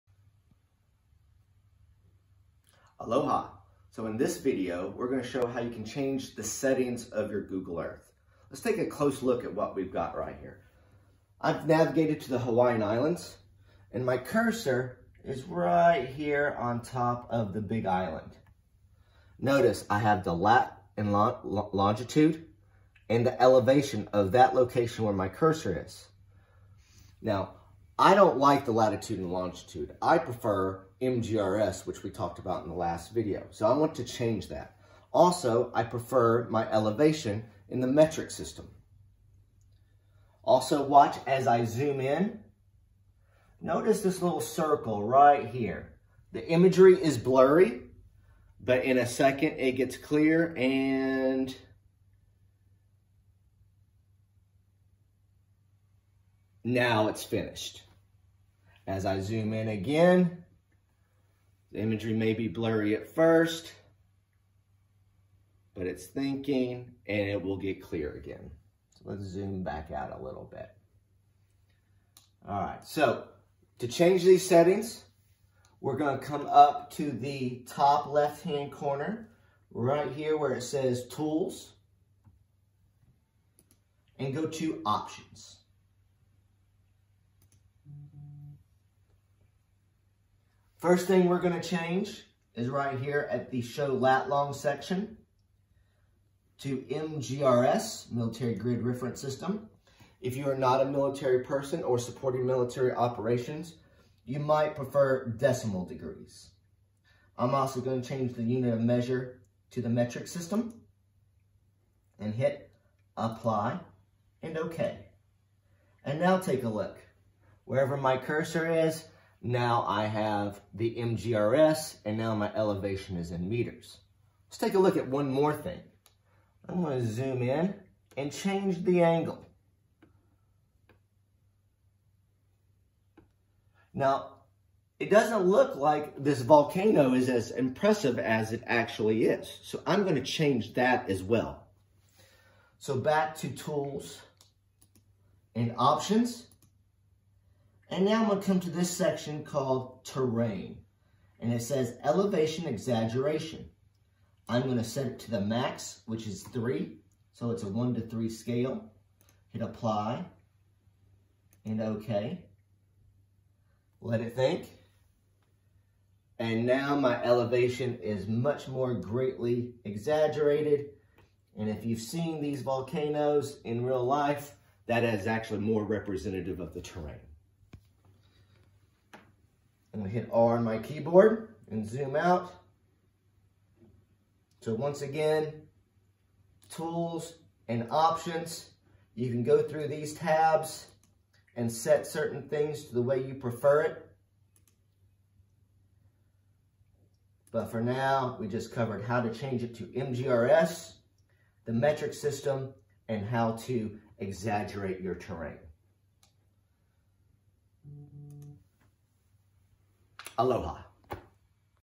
Google Earth Basics 4: Setting Options
Aloha, how to change Google Earth settings from tools, options.